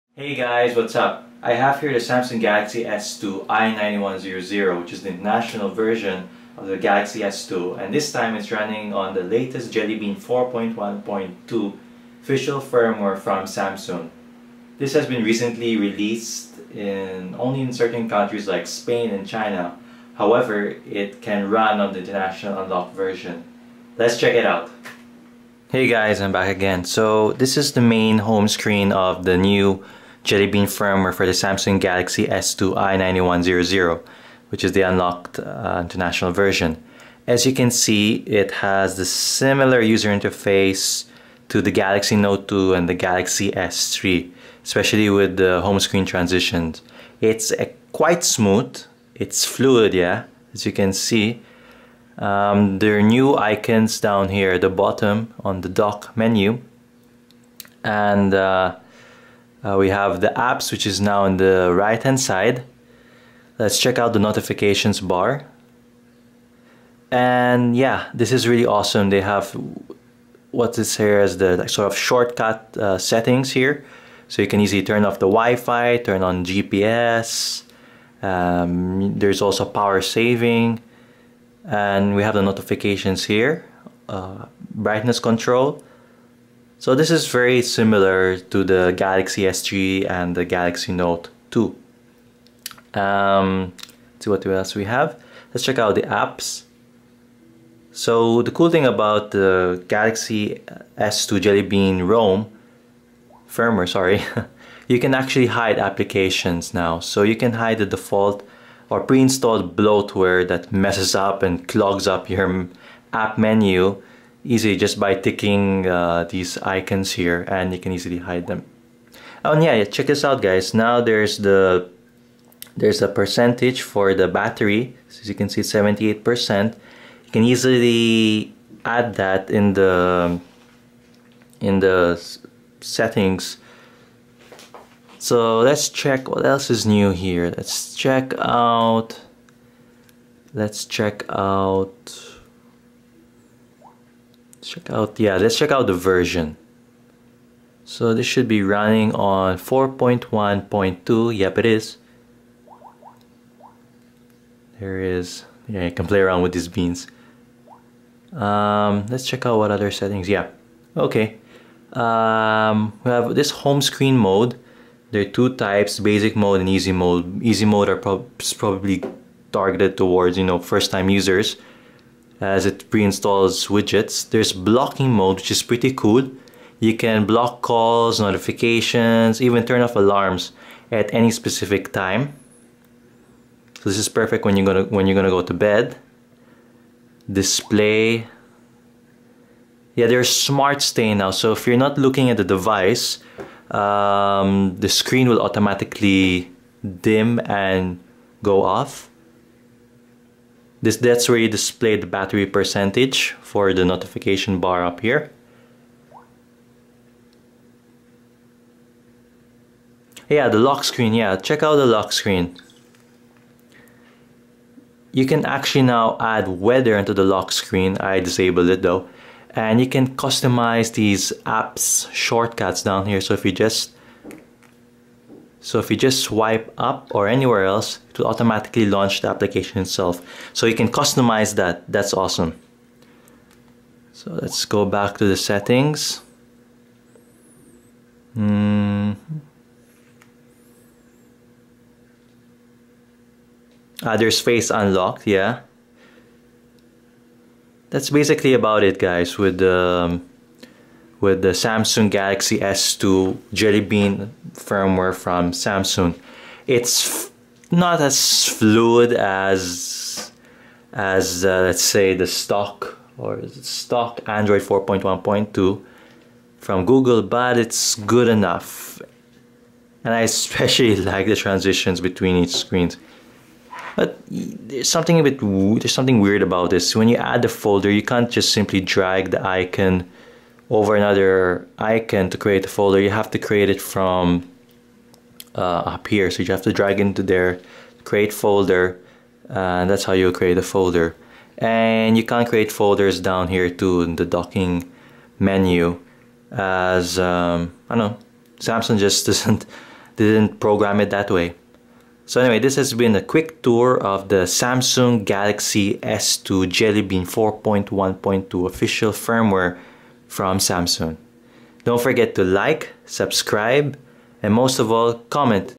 Official Samsung Galaxy S2 i9100 Jelly Bean 4.1.2 Firmware [XWLS8]
Send me on a 5 Day Trip to Computex 2013 in Taiwan

Updated Guide on Flashing Spain's Firmware on any Samsung Galaxy S2 i9100

Samsung officially released the Jelly Bean (4.1.2) Firmware for the Galaxy S2 i9100 (International Version) to two selected countries, Spain and China. Both firmwares are compatiable to most i9100 devices, thus, if you purchased an unlocked Galaxy S2 i9100 from another country it would work flawlessly with Spain's Jelly Bean firmware.

What's New with the Official Firmware 4.1.2 for Galaxy S2 i9100:

- Updated TouchWiz User Interface - Similar to Galaxy S3 and Note 2 (Smoother -Transitions - Project Butter)
- Added Battery Percentage at Notifications Bar
- Added Smart Stay
- Improved Lock Screen - Custom Apps, Weather and News Stream
- New Galaxy S3 and Note 2 Widgets
- Added Notification Settings Shortcut Drawer